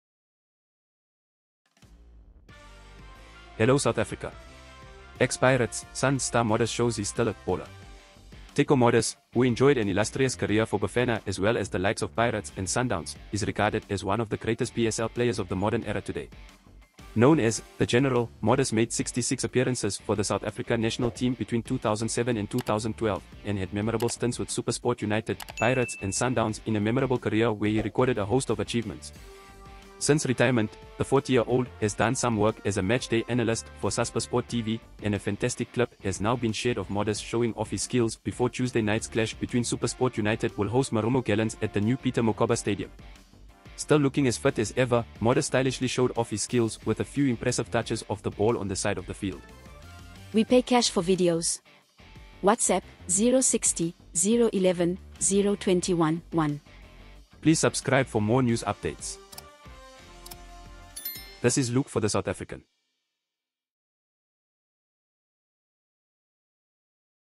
Ex-Pirates, Sundowns star Modise shows he’s still a ‘baller’
Teko Modise, who enjoyed an illustrious career for Bafana as well as the likes of Pirates and Sundowns, is regarded as one of the greatest PSL players of the modern era today.

Video: Twitter/PSLOfficial